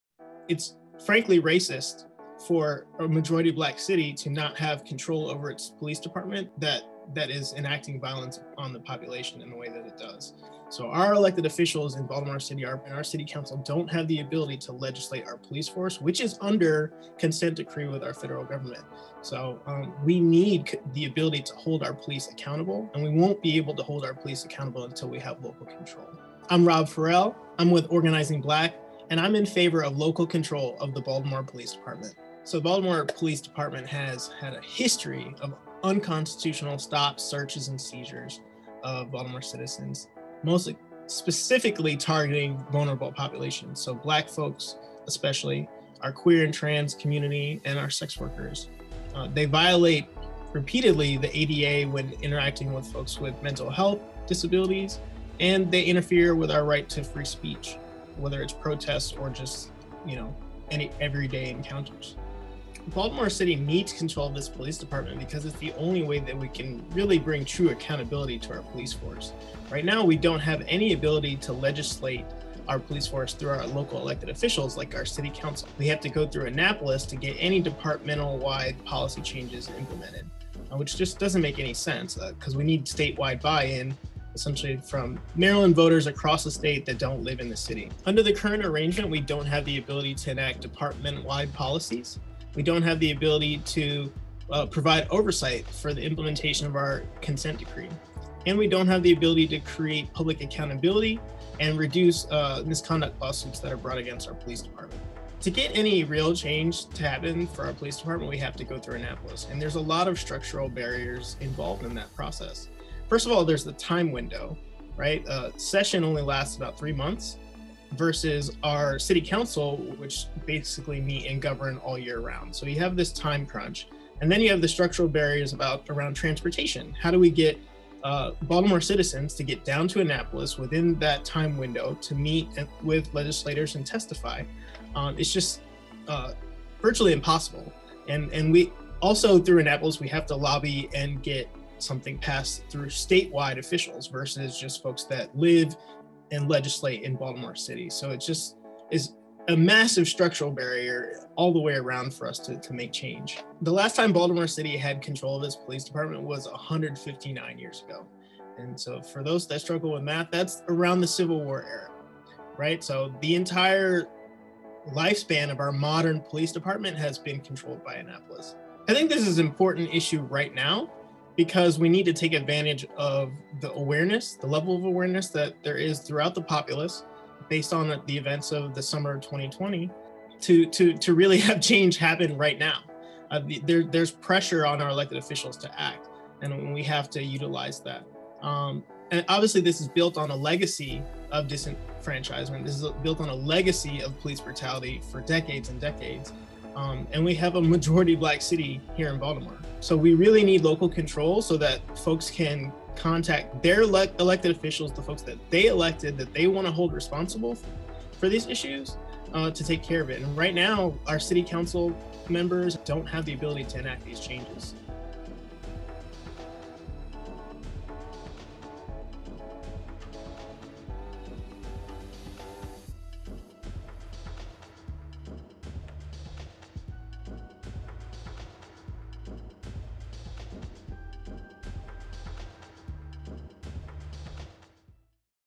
Baltimore Needs Local Control of it's Police
Baltimore City, a majority Black jurisdiction, is the only locality in Maryland that does not govern its own police department. Because the Baltimore Police Department is controlled by the state, Baltimore residents must go to Annapolis to urge legislators to make changes to the policies and practices of their local police department. This must end.